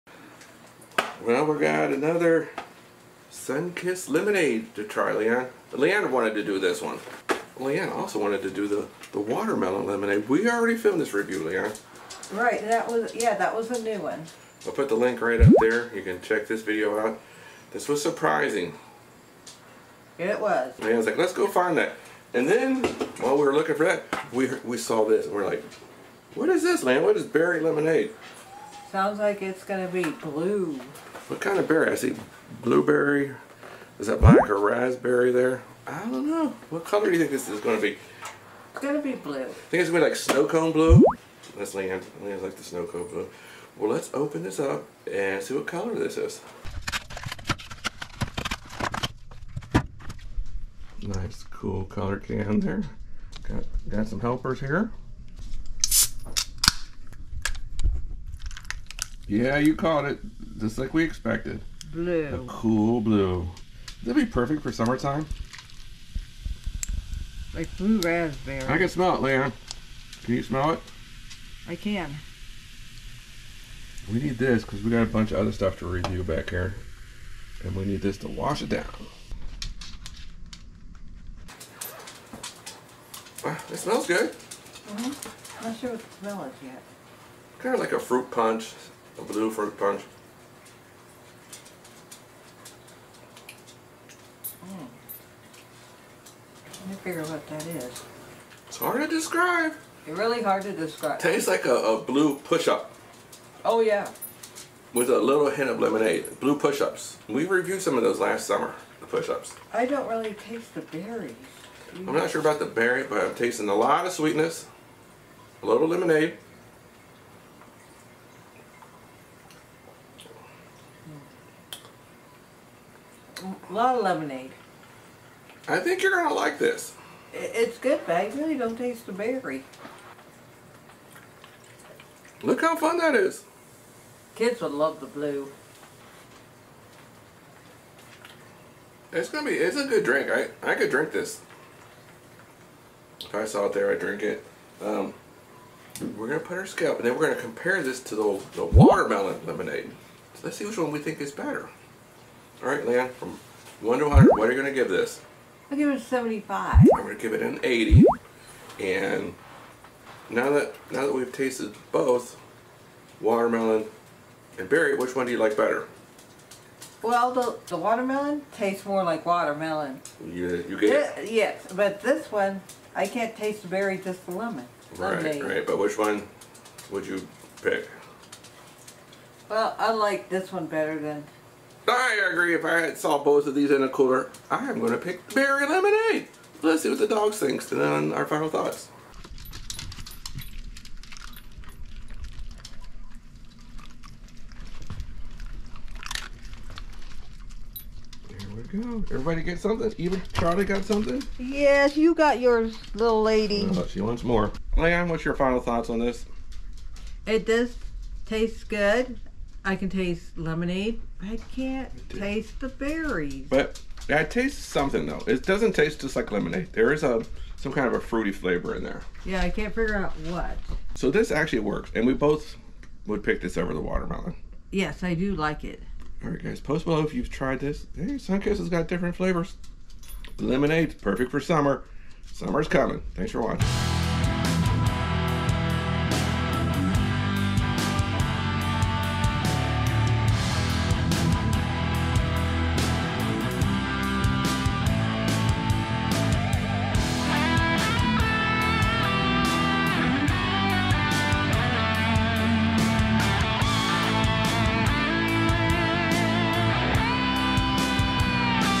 Sunkist BERRY Lemonade Real Review 🫐🍋 - a FUN Summer Drink?
Sunkist Berry Lemonade is a refreshing and fruity twist on traditional lemonade. It combines the tartness of lemonade with the sweet and tangy flavor of mixed berries, creating a unique and delicious taste. The drink is made with real sugar and natural flavors, and contains no artificial colors or preservatives.
PLEASE help to SUPPORT Us - CLICK that THANKS Button under the video!